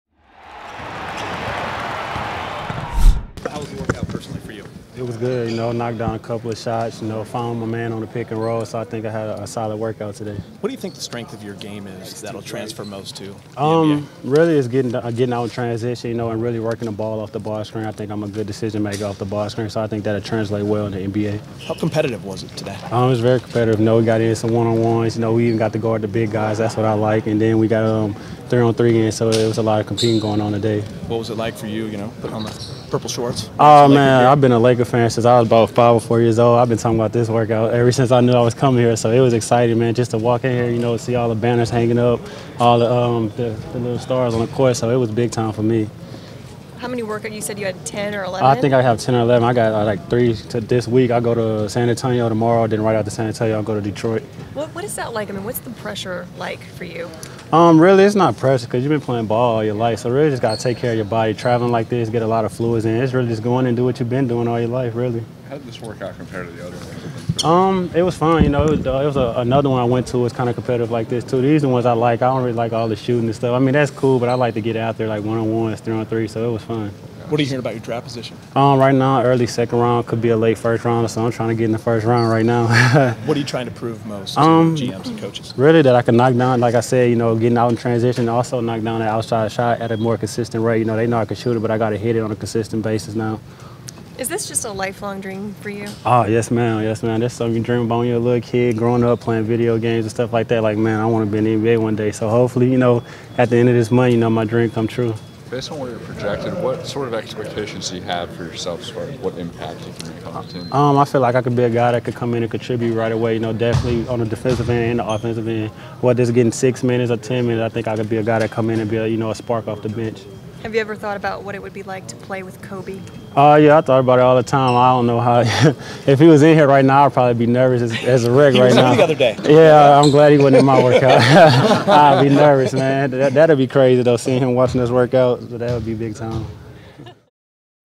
Lakers Pre-Draft Workout: Terran Petteway
The Los Angeles Lakers worked out Terran Petteway out of Nebraska.